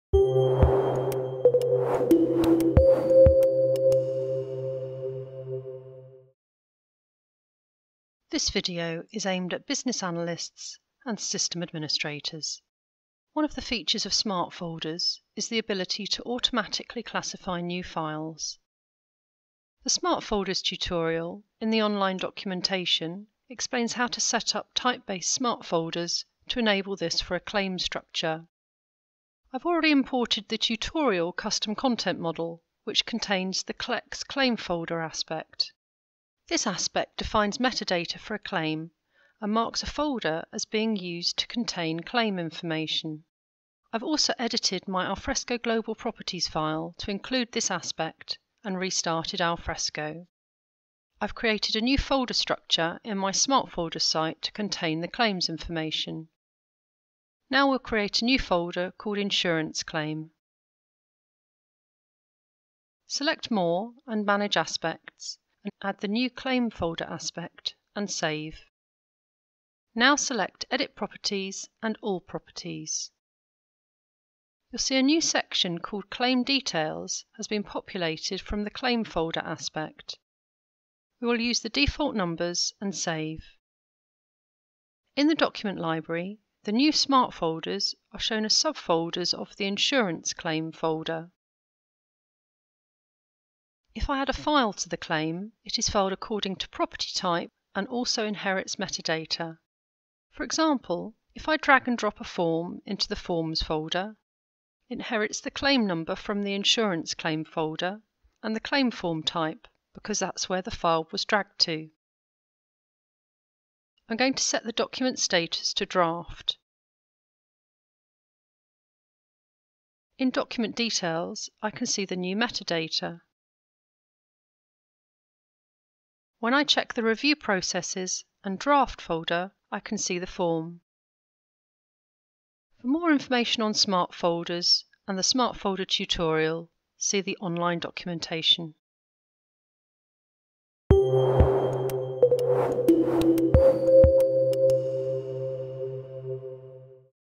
Alfresco 5.1 - Metadata Inheritance with Smart Folders
This Alfresco One video shows you how to create user groups in Alfresco.